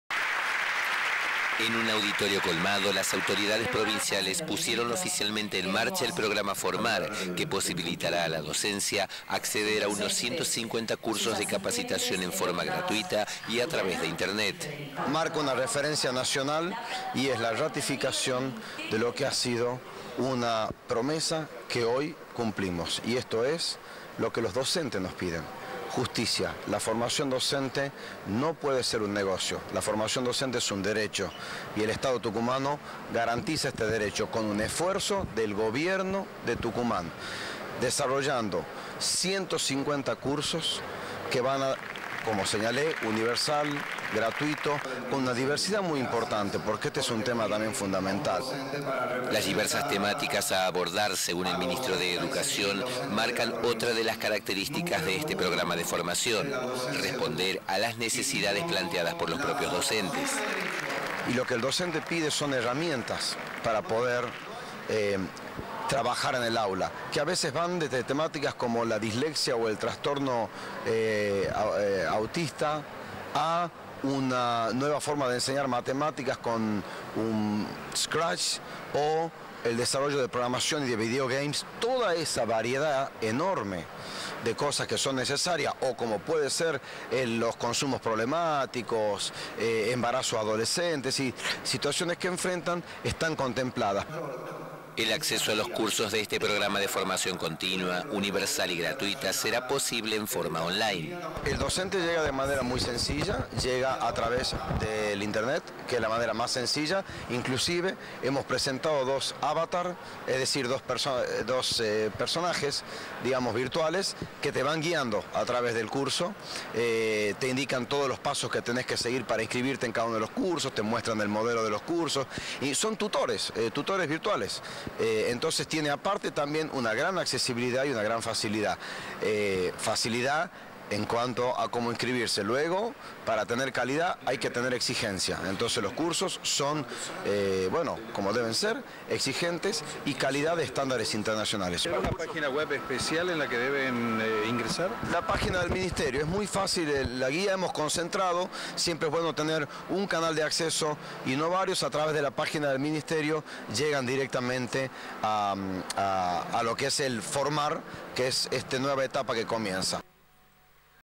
Cursos de formación docente
El ministerio de educación de la provincia puso oficialmente en marcha hoy un programa de formación continua, universal y gratuita destinada a docentes de los niveles primarios y secundarios. A este programa, denominado "formar", los maestros podrán acceder de cualquier rincón de la provincia a través de internet.  Abordara distintas temáticas y otorgara puntajes.